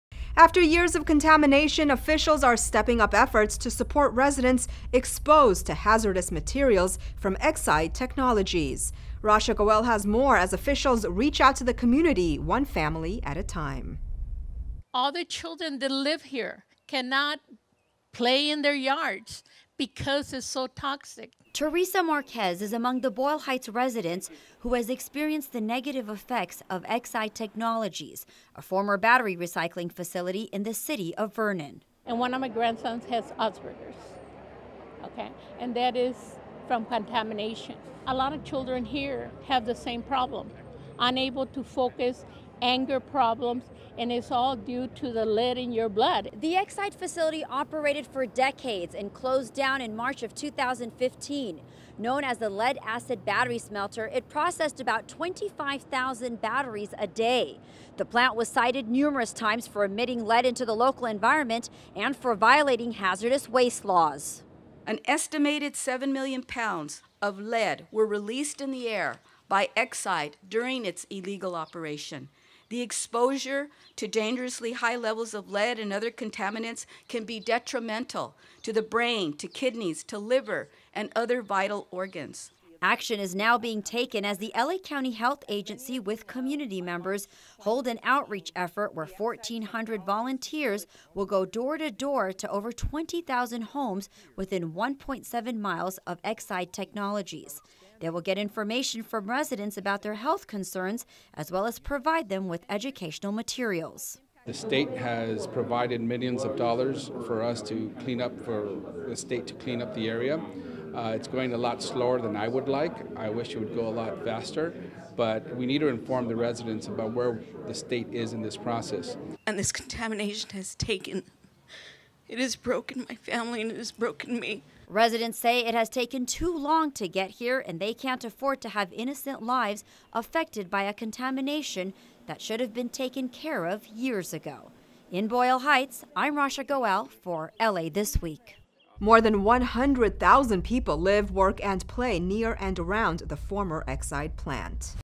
Community Outreach As Result of Exide
Many lives have been affected due to the contamination from Exide Technologies.  LA County Health Agency and community members are working together with volunteers going door to door getting information from residents about health concerns and providing education materials.  Rasha Goel reports
Cameraman: Enrique Roman